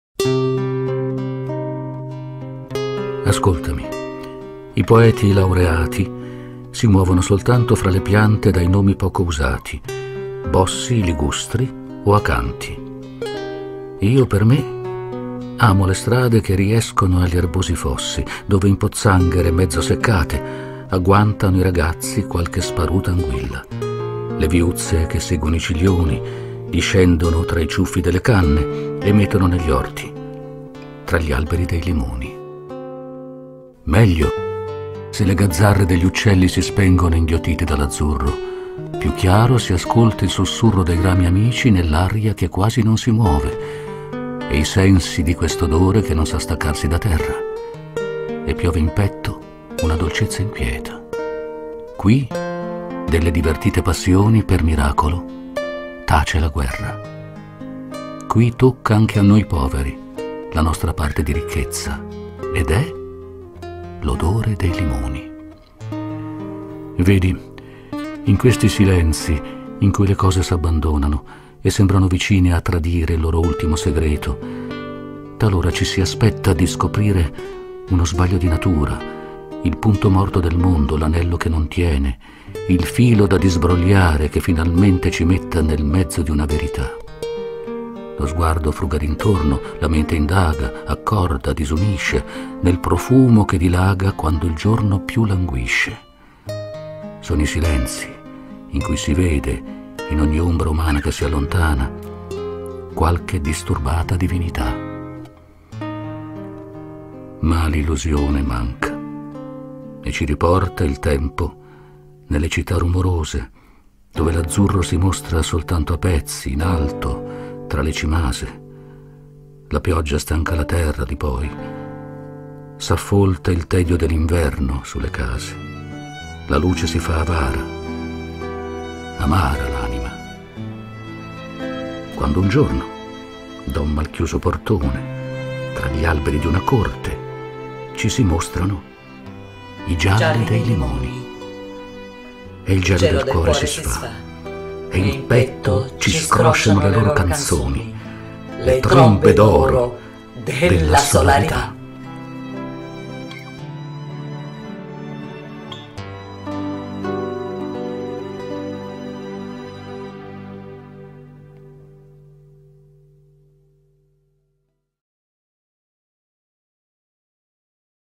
I limoni Eugenio Montale di silvia nascetti
Poesia I LIMONI di Eugenio Montale, recitata da Nando Gazzolo
video realizzato da Silvia Nascetti
fondatrice del gruppo Facebook
DONNE CHE SI SONO STESE SUI LIBRI E NON SUI LETTI DEI POTENTI E...UOMINI CHE LE AMANO COSI'